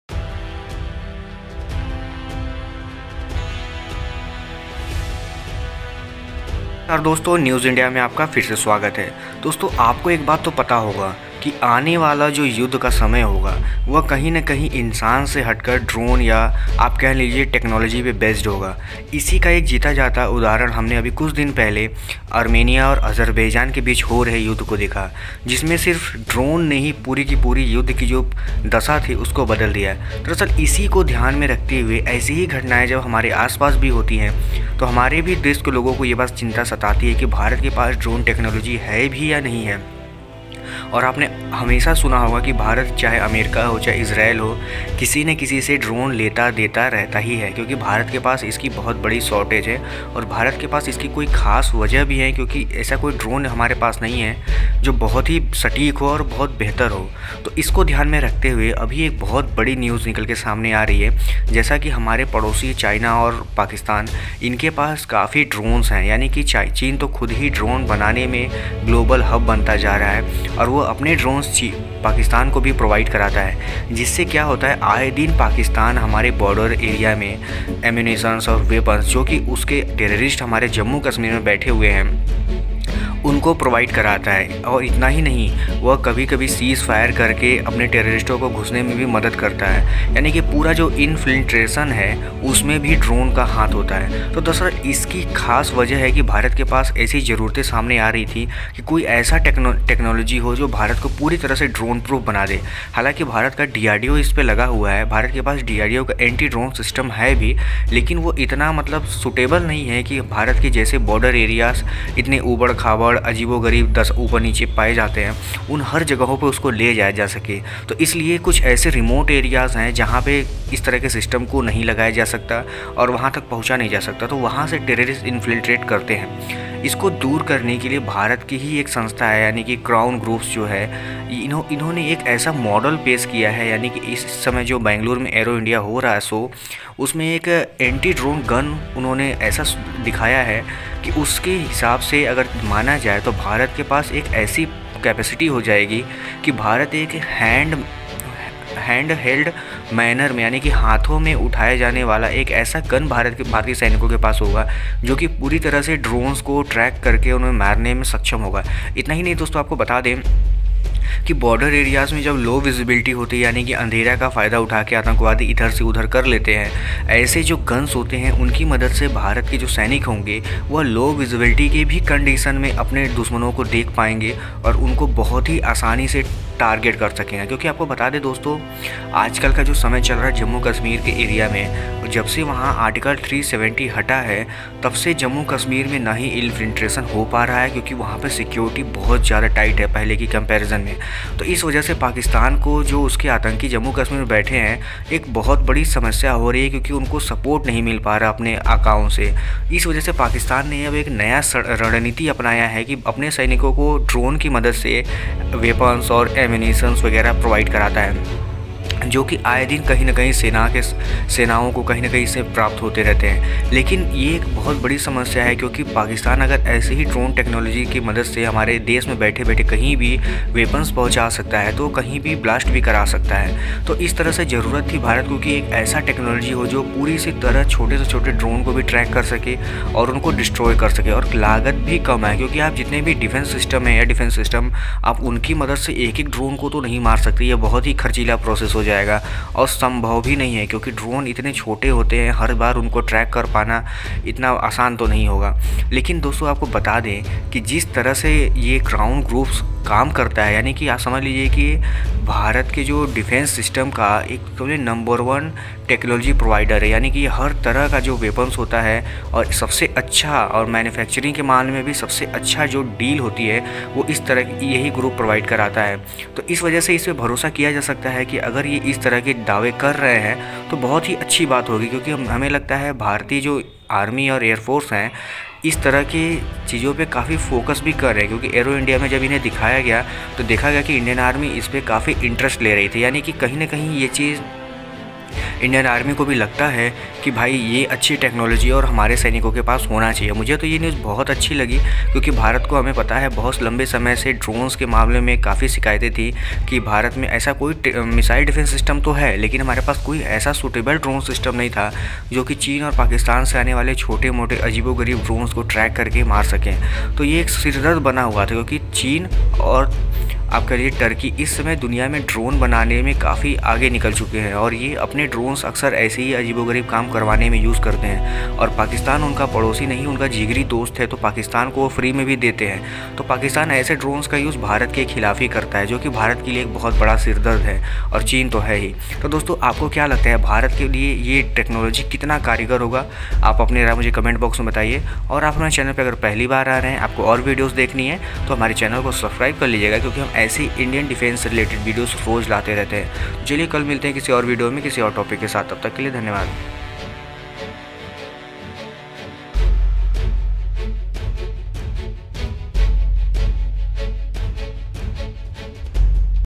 Crown group shown the Anti drone gun in Aero India | News India
Anti drone gun

Description

According to recent reports, the Indian Army has shown interest in the Anti-Drone gun developed by the Crown group. Anti-drone gun has been in demand for a very long time as it provides a solid defence against Drone supplies which are carried out by Pak army to support terror outfits operating in India.

Teown Group is the number one technology provider, component manufacturer, and strategic partner in India’s defence sector. The group has developed an Anti-Drone gun that can be operating in a hand-held manner while performing operations along border areas with Pak and China.

An Anti-drone gun has been in demand for a very long time by Indian forces deployed alongside LOC and LAC as both Pakistan and China have crossed the Indian border on multiple occasions using their drones. Crown Group recently unveiled its Anti-Drone gun at Aero India 2021, which is currently taking place in Bangalore.

Drones can be used to collect sensitive information, supply weapons, and carry precision strikes across enemy territory. Indian Army already has Anti Drone systems Developed by DRDO that can spot multiple drones but these systems are not hand-held and hence cannot be deployed in remote areas.

Ever since the abrogation of Article 370, security on the LOC has been tightened up by Indian security forces, this has made it difficult for Pakistan to infiltrate terrorists across the border, in response to this, Pakistan has been sending weapons & ammunition to terrorists operating in J&K and Punjab using their drones.

These operations are usually carried out at night when the visibility is low, hence it becomes difficult to spot them with naked eyes, anti-drone gun consists of technologies that can spot and destroy drones even in low visibility. Induction of this Anti-drone will certainly help strengthen India's defences along its borders as well as promote the 'Make in India' initiative.